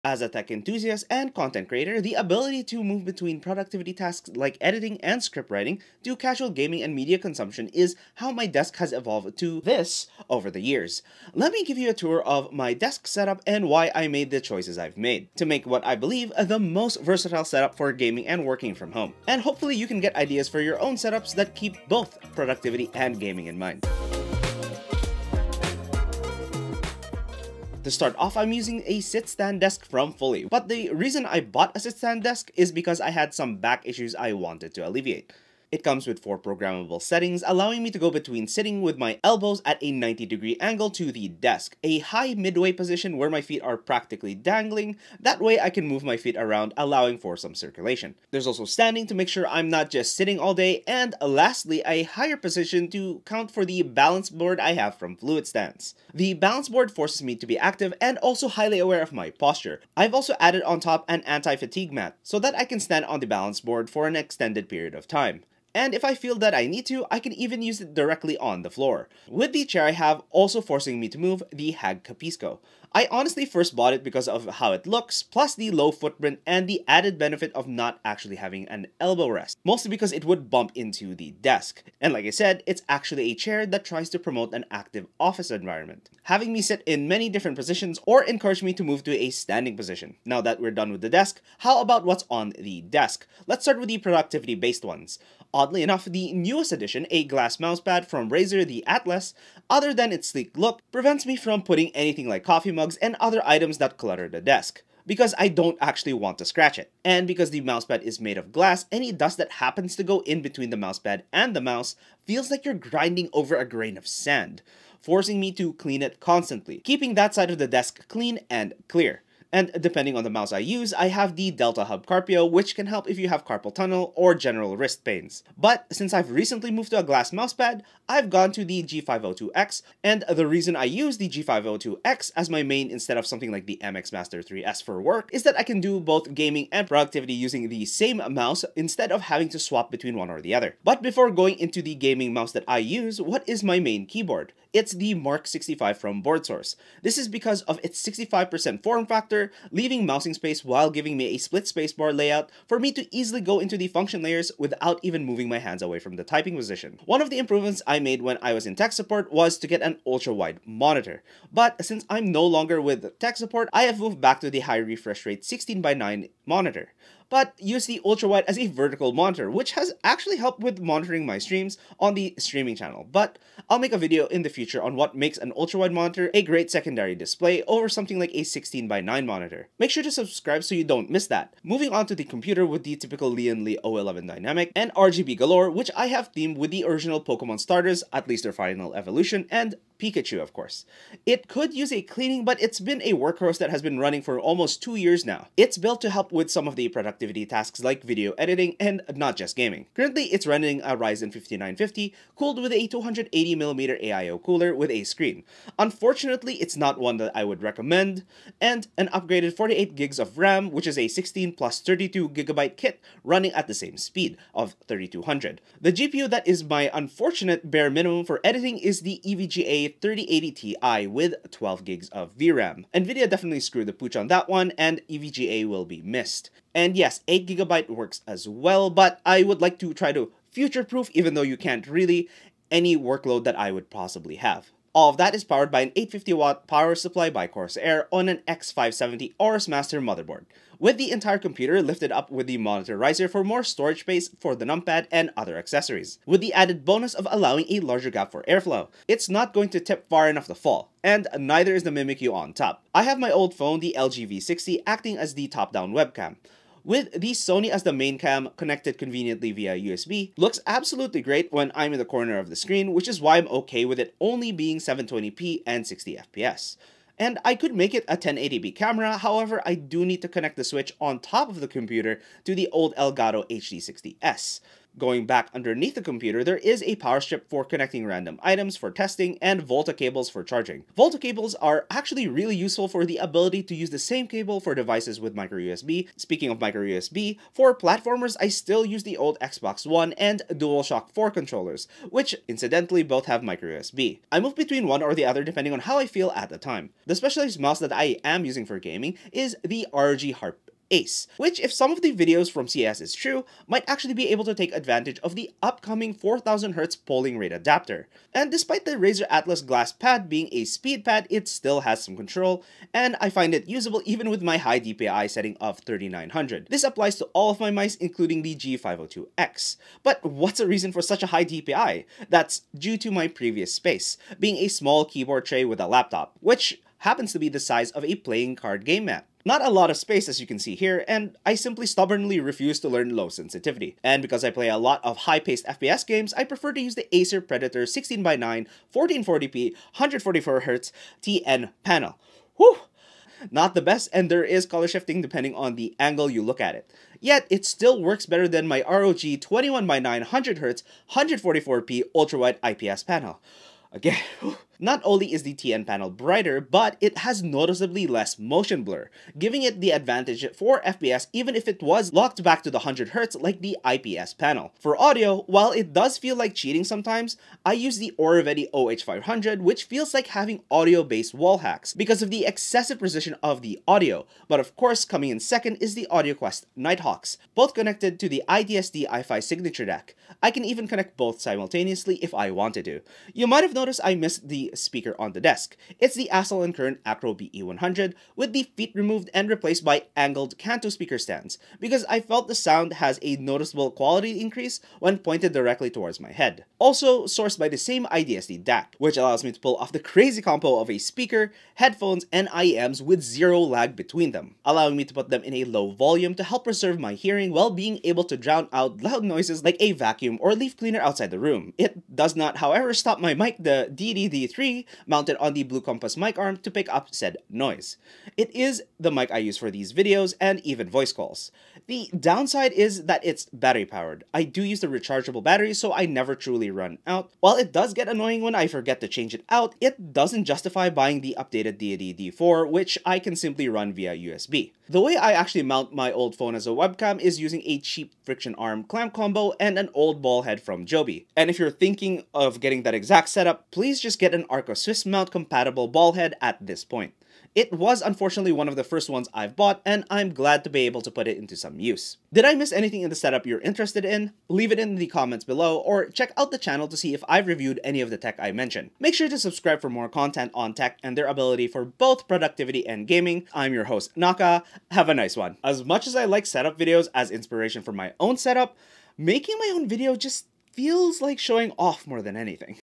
The Ultimate Desk Setup for Gaming & Productivity Tech Enthusiasts
Doubling as a high end editing desk setup, Streaming desk setup and competitive gaming desk setup. My take on a tech enthusiast & content creators desk setup built for both Working from home & gaming. Filled with tech peripherals that will hopefully inspire you to create your own setups.

Amazon Storepage

Desk Accessories:
The Level Balance Board
The Plane Balance Board
Deltahub Wrist rest

Mic and Monitor Arms:
Samson MBA48-48 Microphone Boom Arm
Compass Mic Arm
Extra Tall Monitor Arm

Gaming and Work Mice:
Logitech G502 X
Rog Harpe Ace
MX Master 3S
Razer Atlas Glass Mouse Pad

Computer Parts Equivalent:
AMD Ryzen 5950X
Gigabyte B550 AORUS ELITE
4070 ti super
Corsair RGB Fan
Corsair RAM
Power supply
Audio source Ifi Signature DAC
Better 360 AIO With Screen

Other Electronics:
Samsung Tablet
Nintendo Switch
Switch Pro Controller
Xbox Controller
Playstation Controller

Stream Items:
Sony ZV-E10 Camera Kit
Deity Microphone V-Mic D4